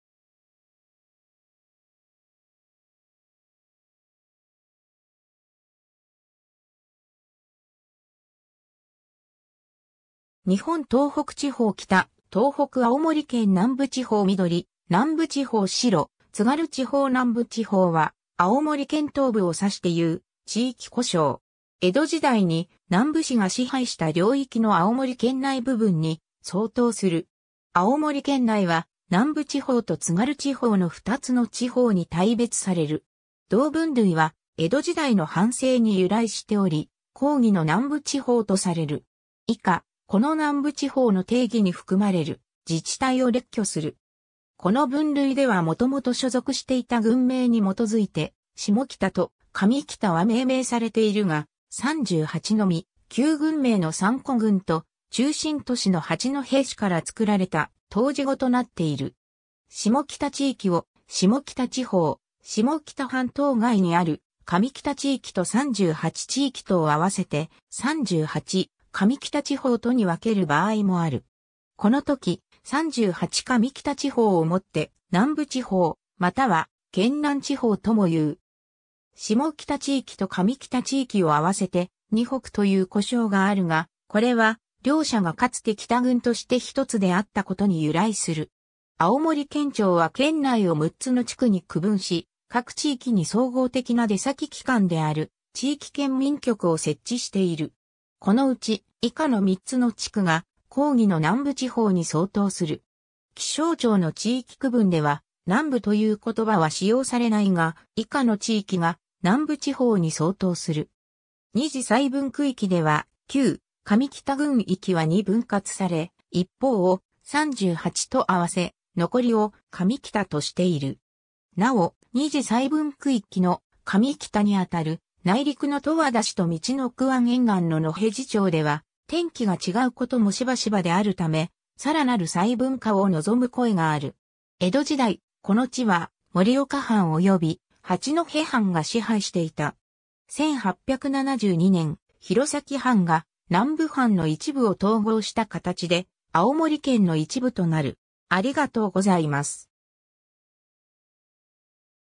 南部地方 (青森県)
日本 」 東北地方 」 北東北 」 青森県 」 南部地方 (青森県) ■緑 ： 南部地方 ■白 ： 津軽地方 南部地方（なんぶちほう）は、青森県東部を指して言う地域呼称。
江戸時代に南部氏が支配した領域（盛岡藩・七戸藩・八戸藩の領域）の青森県内部分に相当する。
 青森県内は、南部地方と津軽地方の2つの地方に大別される。
同分類は江戸時代の藩政に由来しており、広義の南部地方とされる（津軽と南部参照）。
以下、この南部地方の定義に含まれる自治体を列挙する。
 この分類では元々所属していた郡名に基いて「下北」と「上北」は命名されているが、「三八」のみ旧郡名の三戸郡と中心都市の八戸市から造られた頭字語となっている。
 下北地域を「下北地方」（域内の推計人口：66,408人）、下北半島外にある上北地域と三八地域とを合わせて「三八上北（さんぱちかみきた）地方」（域内の推計人口：469,274人）とに分ける場合もある。
このとき、三八上北地方を以って「（狭義の）南部地方」または「県南地方」とも言う。
 下北地域と上北地域を合わせて「二北」という呼称があるが、これは両者がかつて北郡として1つであったことに由来する。
 青森県庁は県内を6つの地区に区分し、各地域に総合的な出先機関である地域県民局を設置している。
この内、以下の3つの地区が広義の南部地方に相当する。
 気象庁の地域区分では「南部」という言葉は使用されないが、以下の地域が南部地方に相当する。
二次細分区域では旧・上北郡域は2分割され、一方を「三八」と合わせ（以下の表中で太字）、残りを「上北」としている。
なお、二次細分区域の「上北」にあたる、内陸の十和田市と陸奥湾沿岸の野辺地町では天気が違うこともしばしばであるため、さらなる細分化を望む声がある。
 江戸時代、この地は盛岡藩および八戸藩が支配していた。
1872年、弘前藩が南部藩の一部を統合した形で青森県の一部となる。